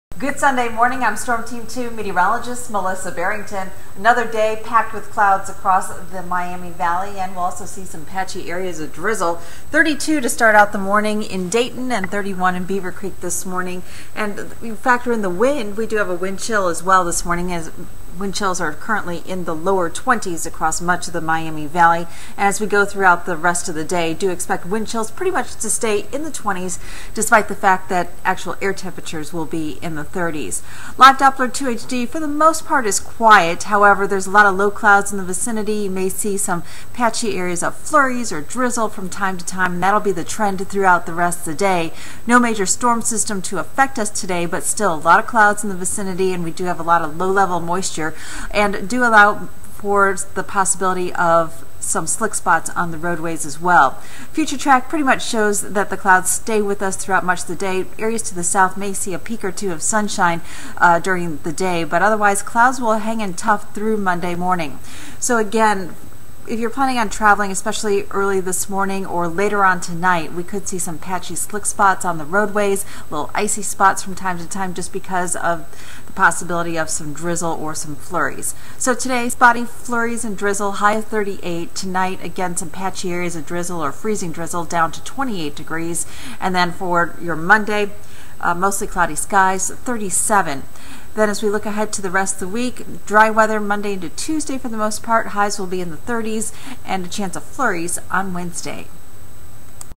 Storm Team 2 Forecast
Cloudy and cold today with a few flurries or drizzle.